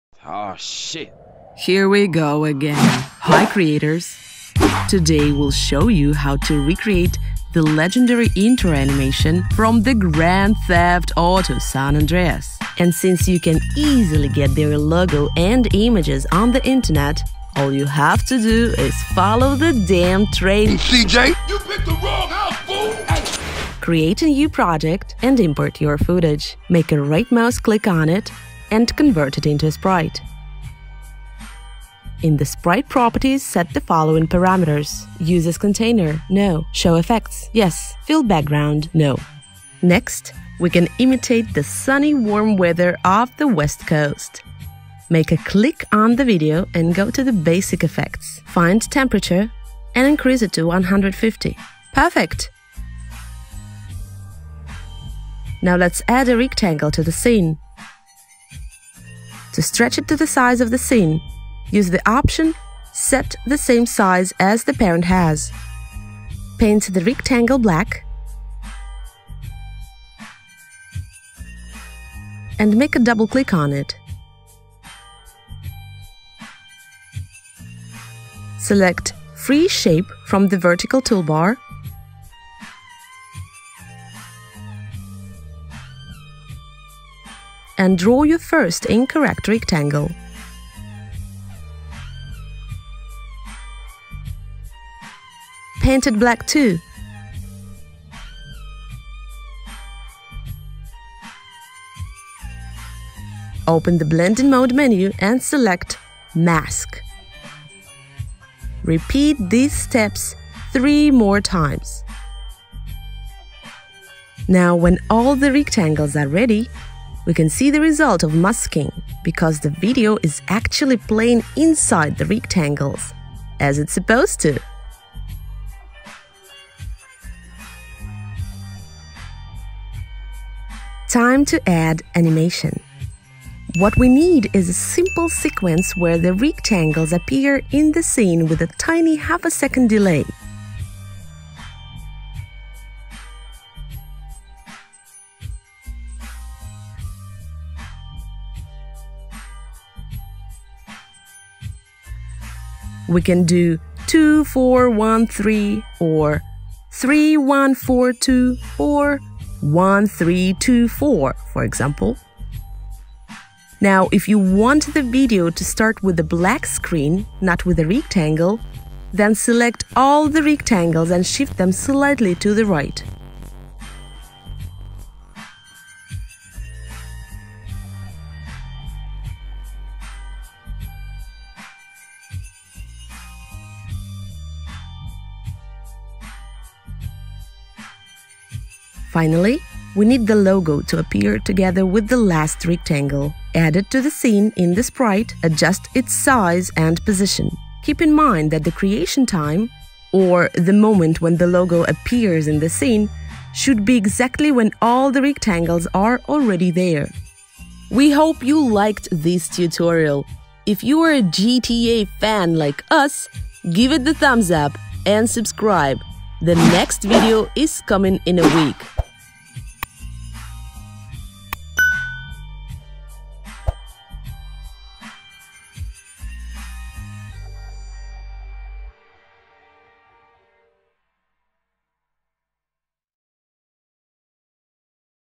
How to make GTA San Andreas intro animation in VSDC Pro
Today, we'll show you how to EASILY recreate the legendary intro animation from GTA San Andreas.

Key tools we're using in this tutorial:
🔧 Basic effects (Temperature) - to imitate the sunny, warm weather of the West Coast
🔧 Free shape - to draw the tilted rectangles for the see-through effect
🔧 Mask mode - to apply the clipping mask effect and make the video play INSIDE the rectangles.(reminder: Mask is a VSDC Pro feature)

Are there any other awesome intro effects we haven't covered yet?